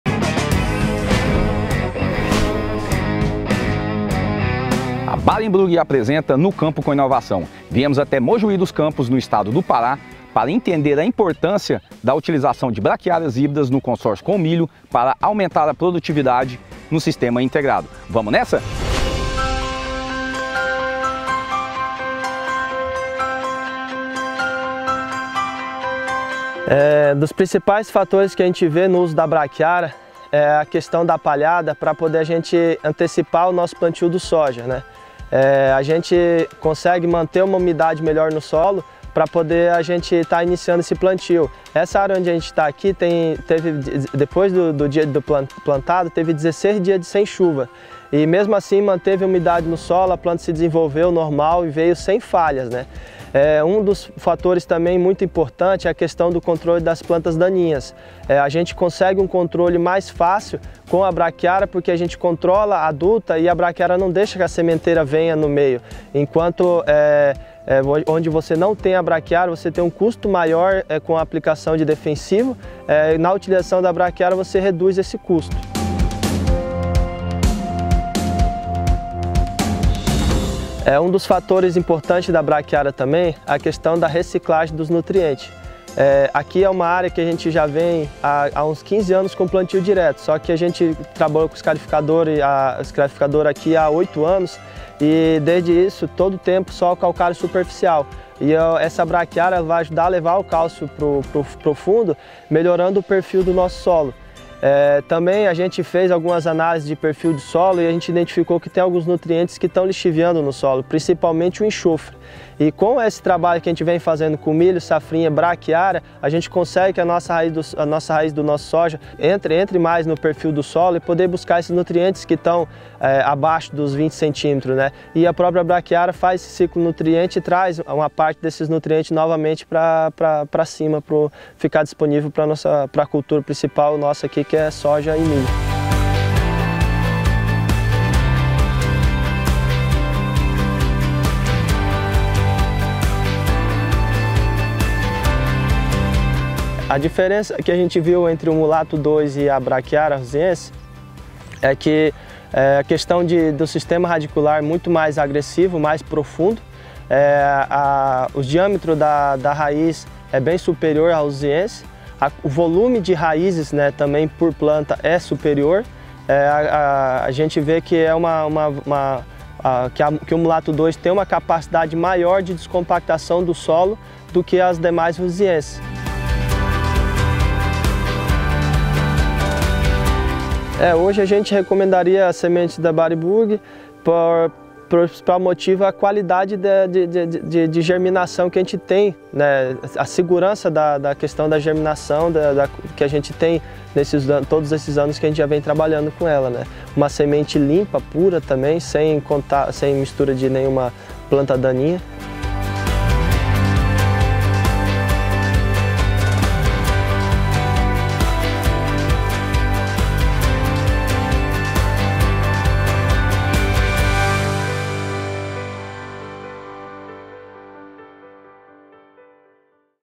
No Campo com Inovação: Fazenda Mato Grosso, Mojuí dos Campos, no Pará
A Barenbrug apresenta o "No Campo com Inovação", com a jornada de sucesso dos irmãos Fernando e Felipe Hermes, ambos proprietários da Fazenda Mato Grosso. A propriedade rural está localizada em Mojuí dos Campos, no estado do Pará, e trabalha com o plantio de soja e milho.

Felipe destaca o uso das brachiarias híbridas da Barenbrug no controle das plantas daninhas. De acordo com o produtor, a opção de soluções tecnológicas trazem um controle mais fácil e com menor custo em comparação com outras variedades de cultivares. “Onde você não tem a brachiaria híbrida da Barenbrug, você tem um custo maior com a aplicação de defensivos”, afirmou Felipe.

Os irmãos recomendam as sementes da Barenbrug pela qualidade, segurança na germinação e pureza.

Conheça toda a história de sucesso dos irmãos Hermes no vídeo completo do novo episódio do “No Campo com Inovação”!